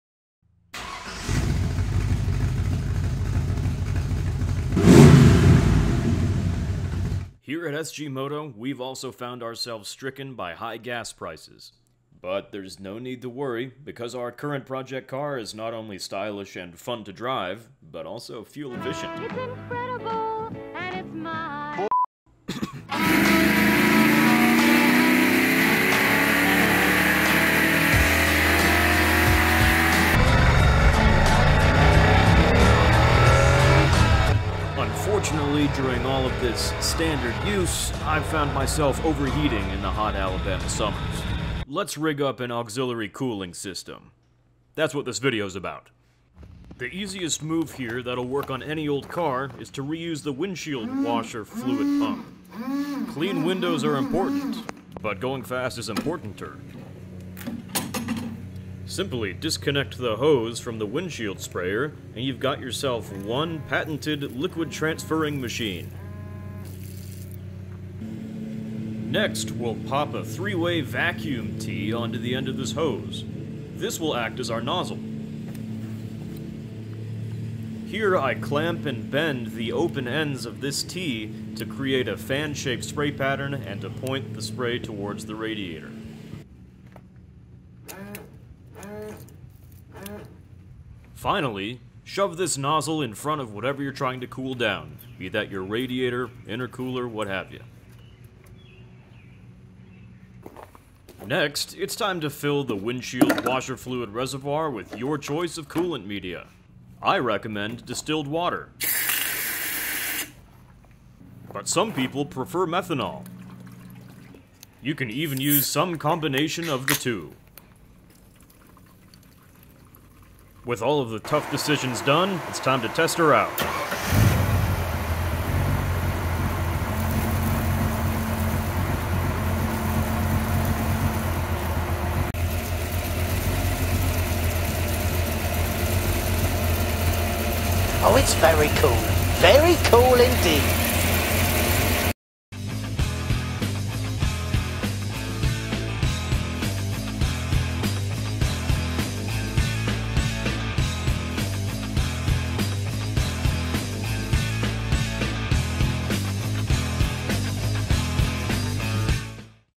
CHEAP AND EASY DIY RADIATOR/INTERCOOLER SPRAY
Is summer bumming you out? Car running too hot while gapping fools around the Wal Mart parking lot?

Look no further! This easy a trick radiator and/or intercooler spray will have you staying cooler than a Polar Pop on a July day!